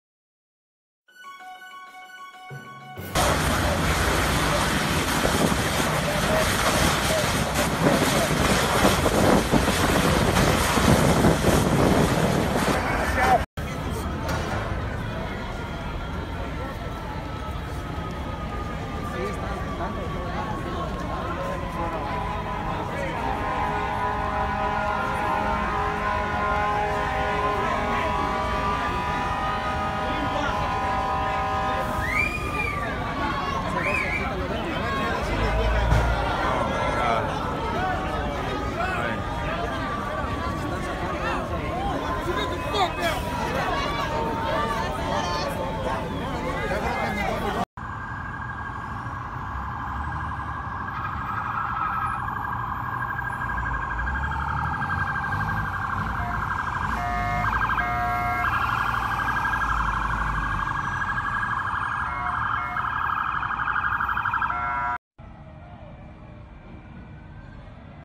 Massive 5-Alarm fire raging in Hamilton Heights!
Video posted on twitter from witnesses to the Hamilton Heights 5-alarm fire this afternoon. More than 200 firefighters were on scene. There were minor injures to firefighters but no civilians or firefighters were seriously hurt.